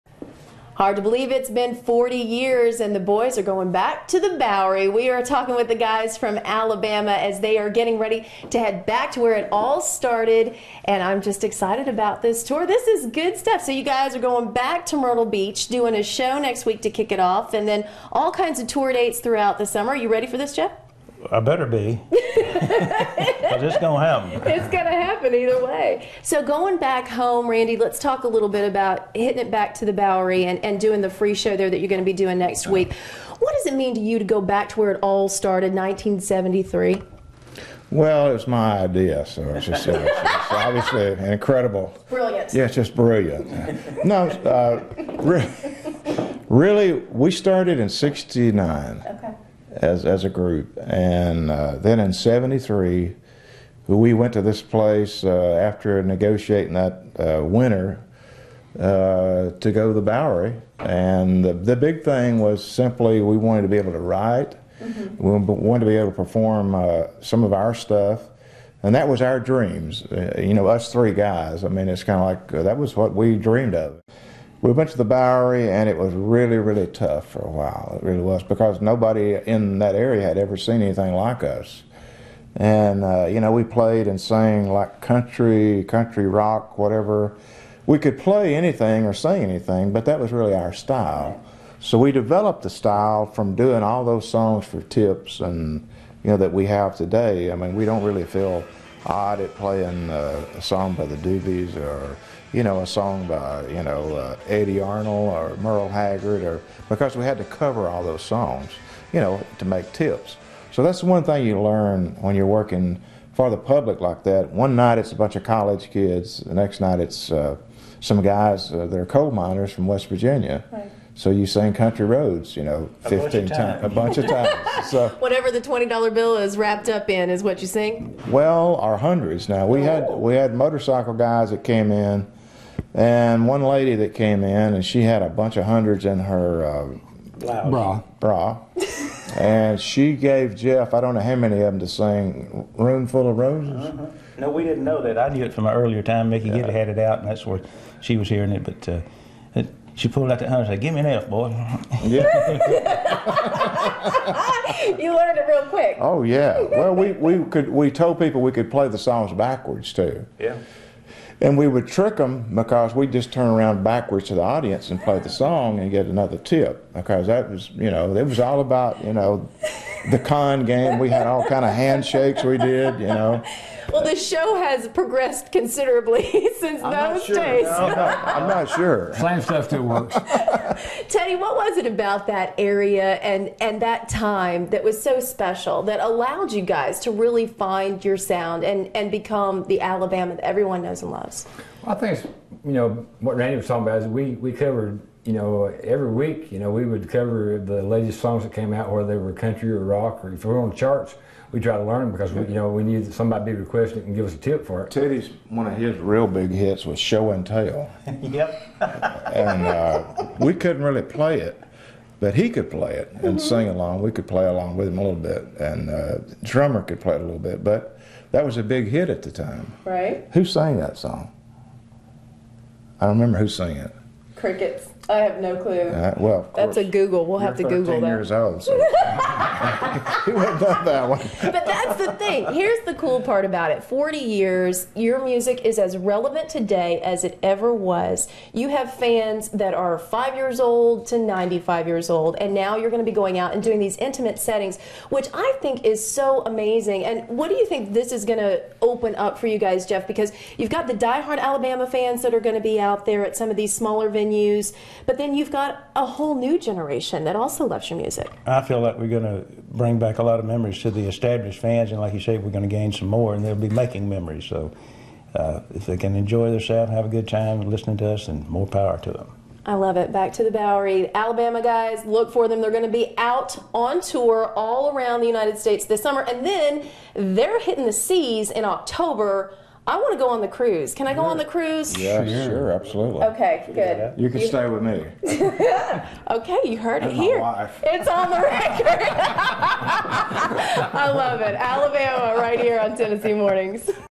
Alabama Launches First Major Tour in More Than a Decade
It's been 40 years since the band Alabama first rocked the Bowery in Mytrle Beach,  South Carolina. Now, dozens of awards and 43 number one hits later,  the group is headed back to where it all began.  On April 5th, the guys are playing a free show at the place they honed their skills before hitting the big time.  Randy owen says it was the early years there that helped develop the alabama sound. So we developed our style from playing all those songs for tips that we have today.  We don't really feel odd doing a song by the Doobie Brothers or a song by Eddie Arnold or Merle Haggard... because we had to cover all those songs to make tips. Next week's show is kicking off a summer run that marks the bands first major tour schedule since 2002. They stop at The Grand Ole Opry on April 23.